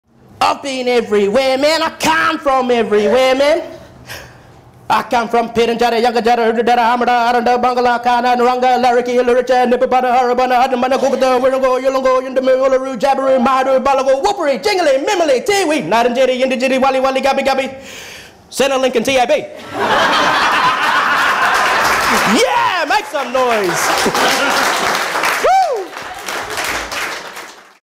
Everywhere Gigolo
Joshua Warrior smashing it at Tandanya, Adelaide Fringe, 2017.
Video by Finton